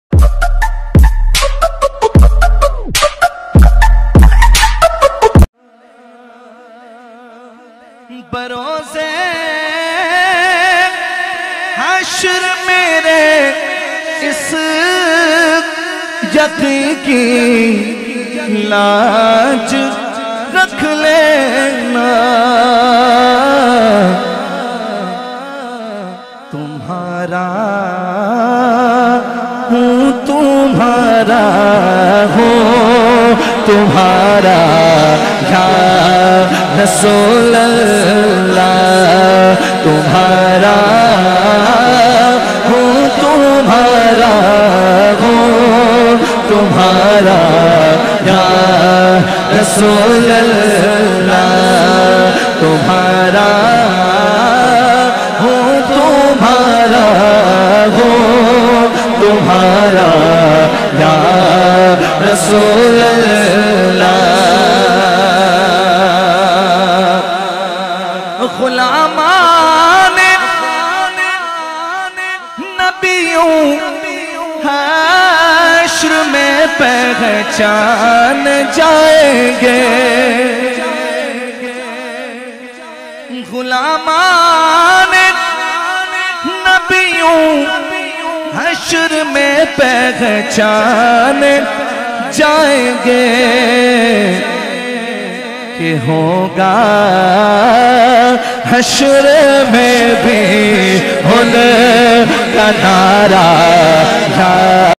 10 Muharam ki Mehfil Mei Humhe Ziyarat Naseeb Hoee (Ap bhe Ziyarat krlein) ♥️🕌
ہم سب کو آج سرکار ﷺ, حضرت امام حسن ،اور حضرت امام حسین کے موئے مبارک کی زیارت نصیب ہوئی الحمدللّٰہ♥️